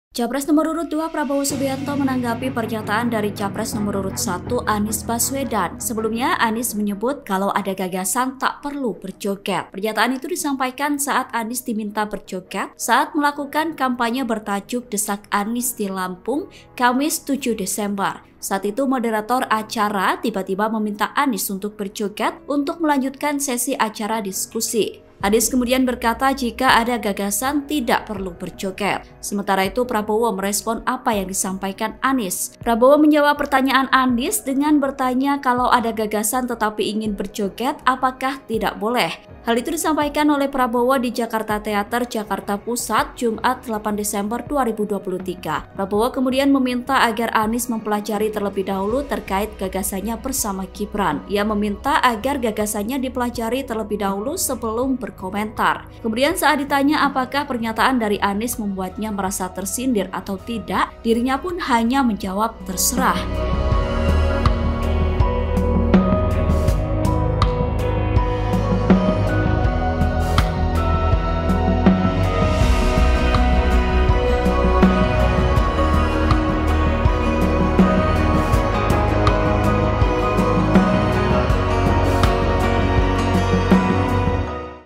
Anies Sebut Jika Ada Gagasan Tak Perlu Joget, Prabowo: Gak Boleh? Pelajari Gagasan Jangan Komentar
TRIBUN-VIDEO.COM - Capres nomor urut 2 Prabowo Subianto menanggapi pernyataan dari capres nomor urut 1 Anies Baswedan.

Sebelumnya, Anies menyebut kalau ada gagasan tak perlu berjoget.

Pernyataan itu disampaikan saat Anies diminta berjoget saat melakukan kampanye bertajuk "Desak Anies" di Lampung, Kamis (7/12).

Saat itu moderator acara tiba-tiba meminta Anies untuk berjoget untuk melanjutkan sesi acara diskusi.

Anies kemudian berkata jika ada gagasan tak perlu berjoget.

Sementara itu Prabowo merespon apa yang disampaikan Anies.

Prabowo menjawab pernyataan Anies dengan bertanya kalau ada gagasan tetapi ingin berjoget apakah tidak boleh.

Hal itu disampaikan oleh Prabowo di Djakarta Theater, Jakarta Pusat, Jumat (8/12/2023).

"Kalo ada gagasan tapi mau joget tidak boleh?," tanya Prabowo.

Prabowo kemudian meminta agar Anies mempelajari terlebih dahulu terkait gagasannya bersama Gibran.

Ia meminta agar gagasannya dipelajari terlebih dahulu sebelum berkomentar.

Kemudian saat ditanya pakaah pernyataan dari Anies membuatnya merasa tersindir atau tidak, dirinya pun hanya menjawab terserah.

"Terserah deh," singkat Prabowo. (Tribun-Video.com/WartaKotalive.com)

Artikel ini telah tayang di WartaKotalive.com dengan judul Anies Sebut Kalau Ada Gagasan Tidak Perlu Joget, Prabowo: Jangan Komentar Sebelum Dipelajari ,
Penulis: Alfian Firmansyah | Editor: Junianto Hamonangan